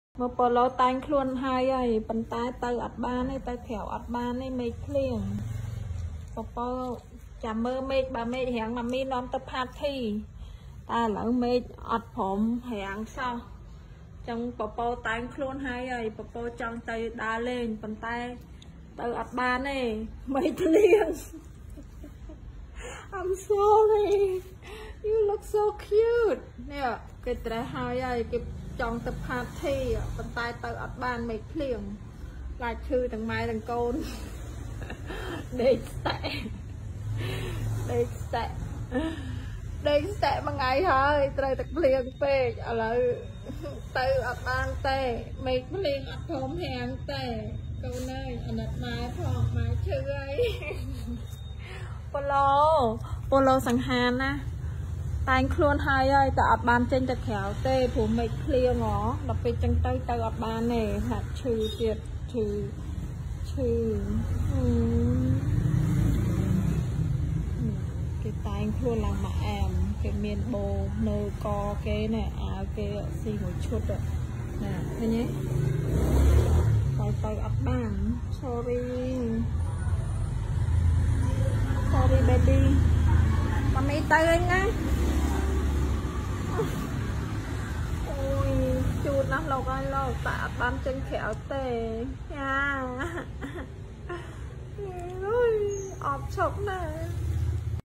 I dressed up my dog 🐶 Cute Dog Gets Dressed for shopping ~ Like for Halloween || Funny Dog 🐶
I'm already dressed for my dog Draw eyebrows​ and Tie the neck in order for the shopping..